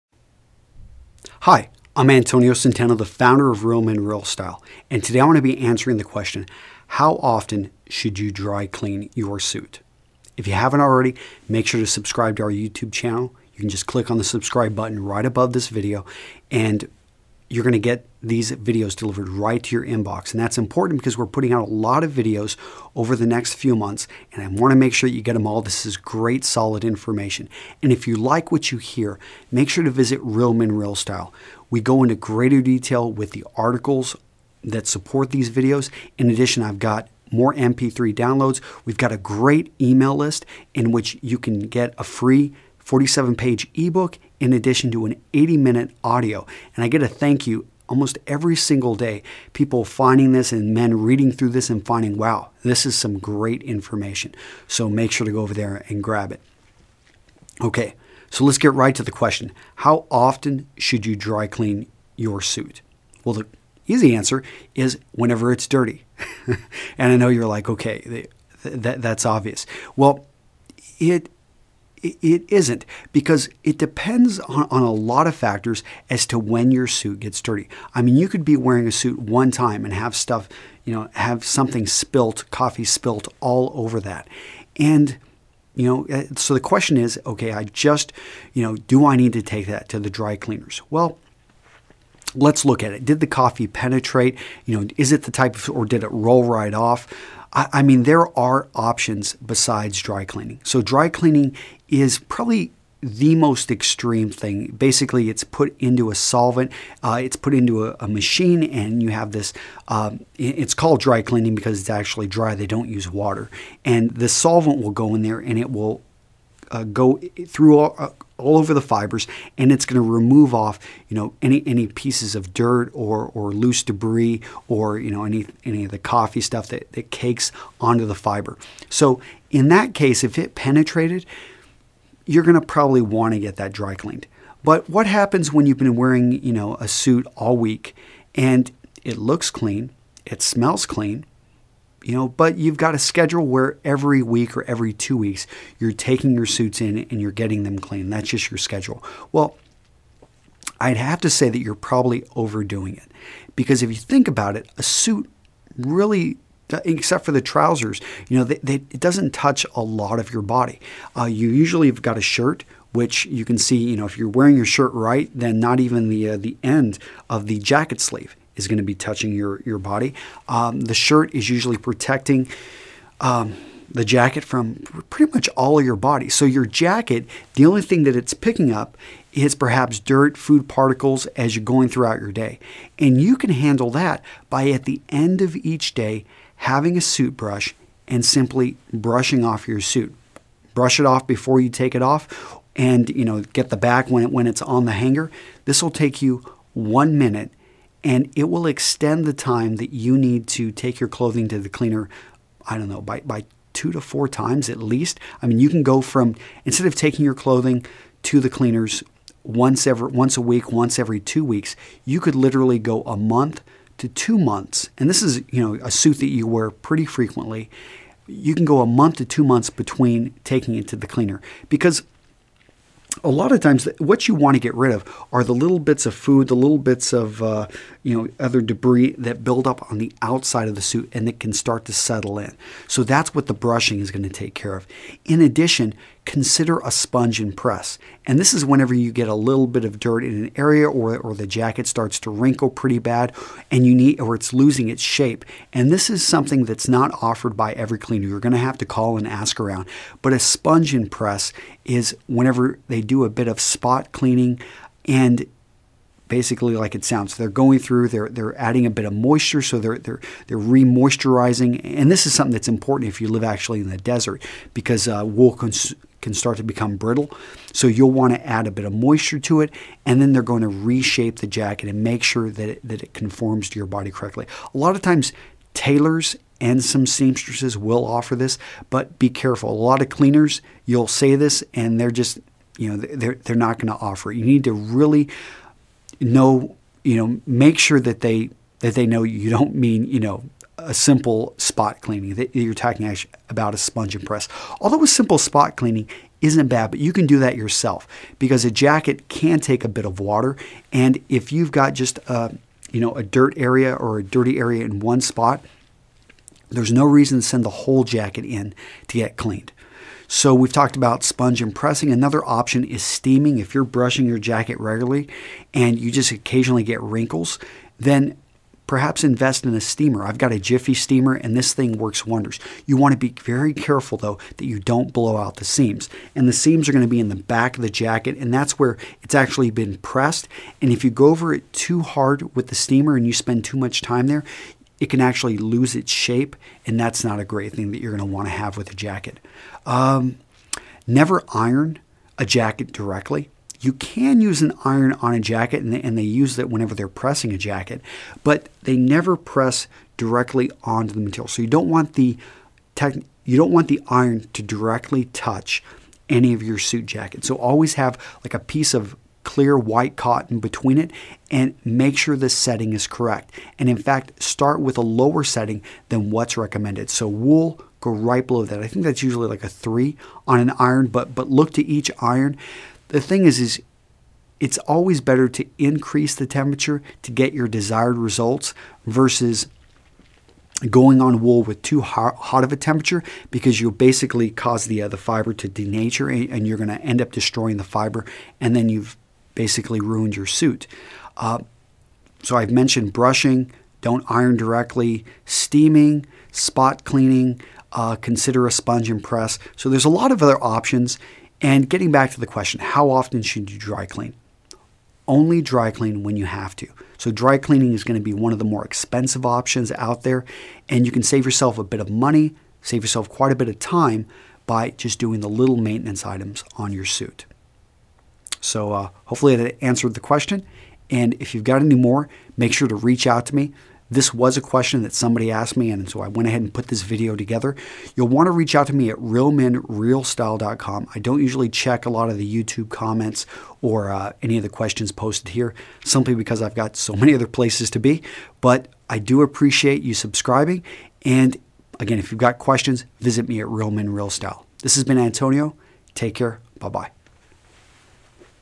Dry Cleaning Men's Suits - When & How Often to Dry Clean Your 2 Piece Mens Suit
- This s a support article on how to brush your suit - you want to do this so you can go longer between dry cleanings.  In this article I also touch on sponge and pressing and spot cleaning and steaming.

Click this link for a FREE 47 page eBook on men's style and fashion.